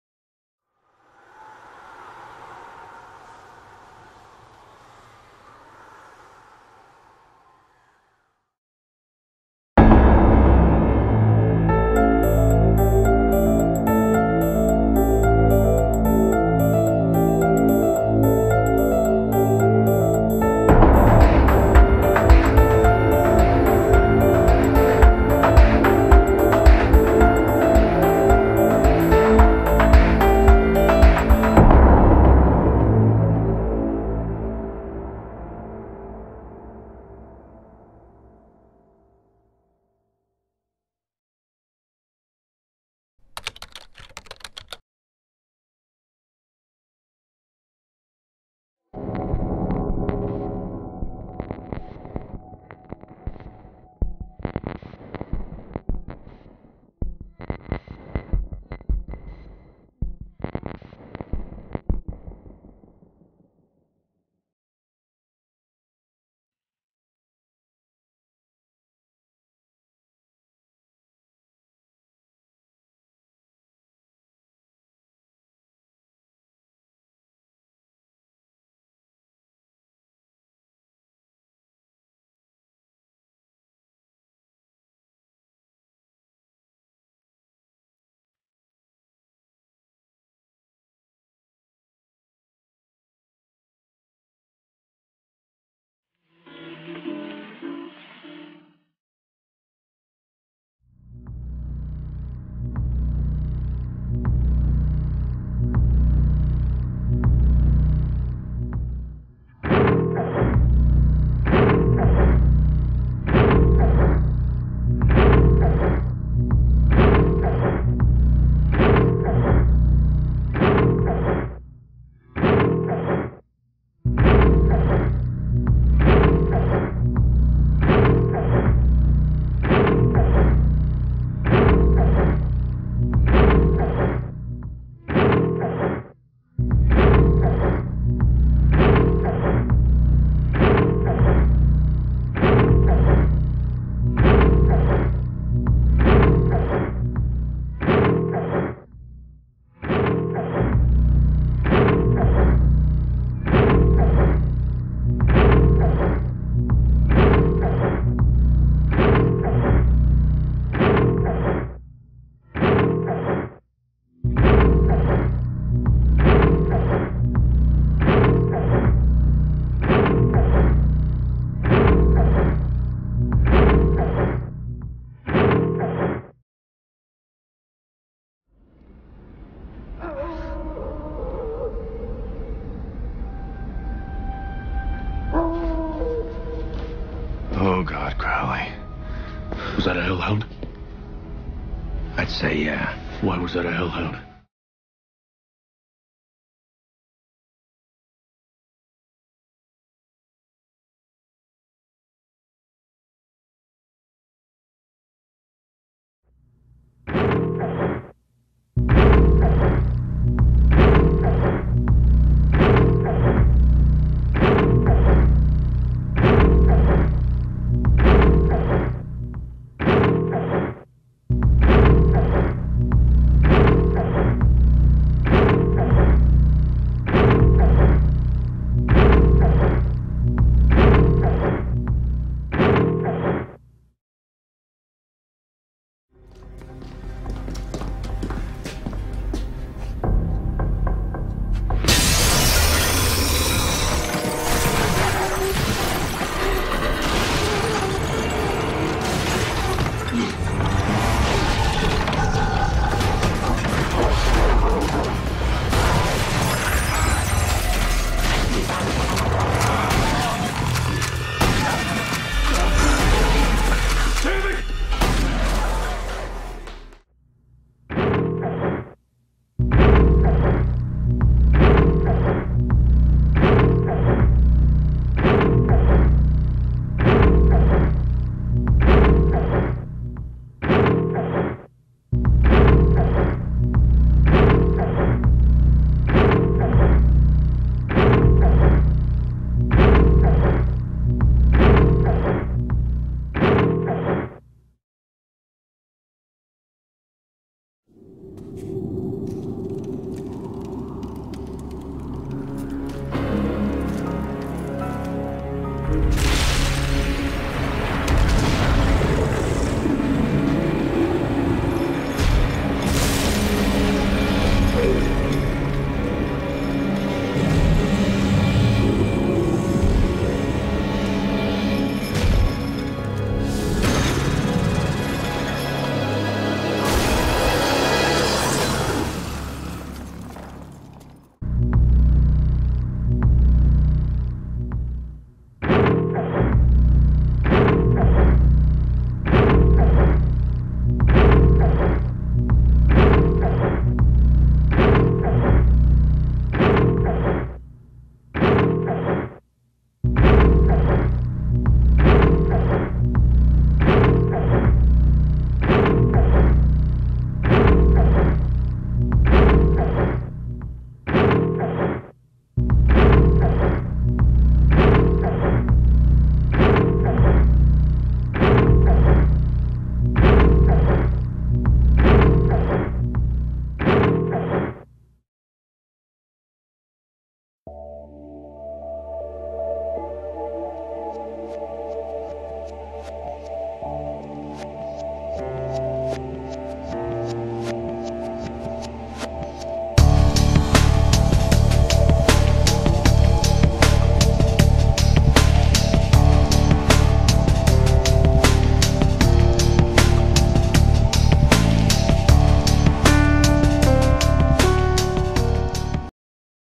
Real Life MYTH:HELLHOUND
These creatures are mentioned in many movies and games but are they real or just mistaken with dogs that have rabies?